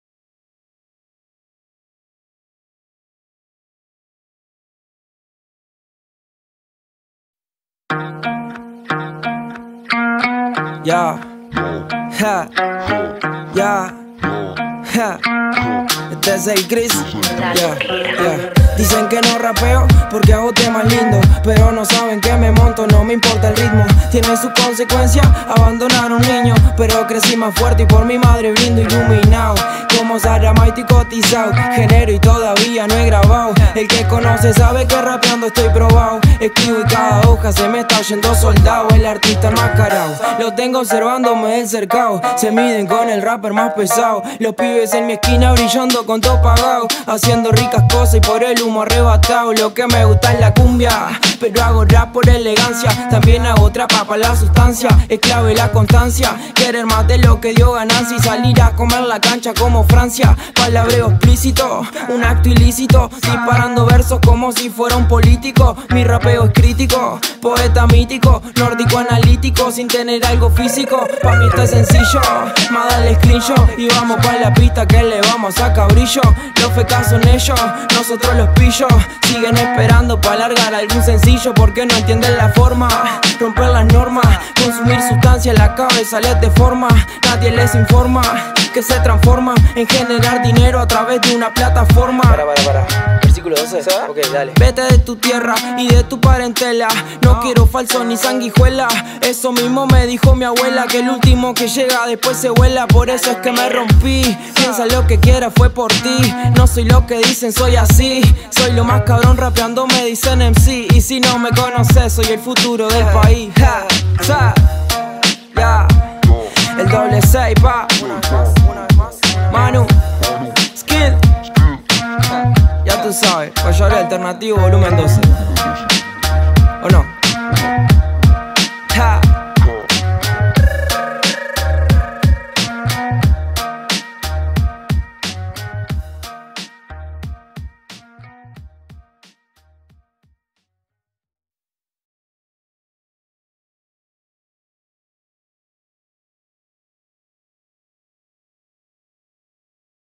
Payadores Alternativos Vol. 12 Cristian Prado
Una producción original de @culturasalternativas con apoyo de @gobiernoderocha y @mec_uruguay @culturamec

Artista: @criss21th

Idea original y Producción Ejecutiva: @mansanmartin
Video: @gonzatfilms y @shottbykevo
Edición: @gonzatfilms
Mezcla: @skilldtr3s